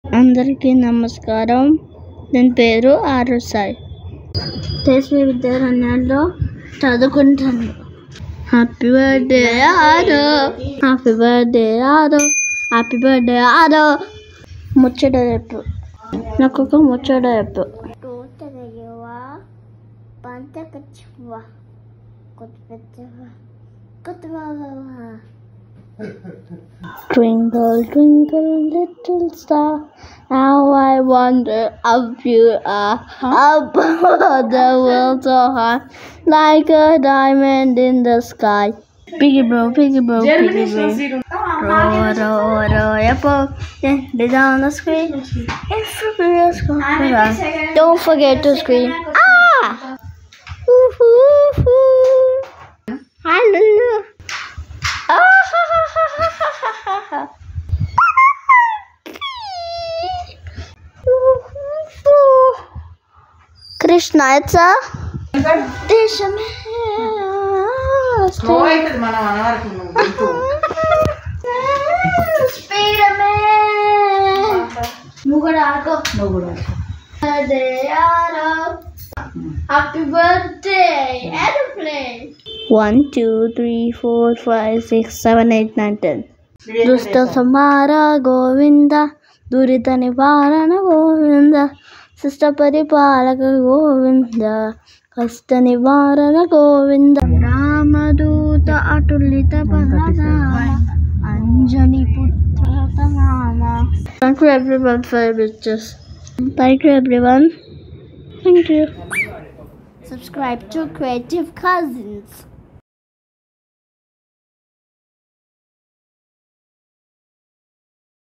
Aarav Birthday Special
Namaste everyone!
One of our cousin, Aarav celebrated his birthday on 15th September, 2024. Here’s a small glimpse of his special moments.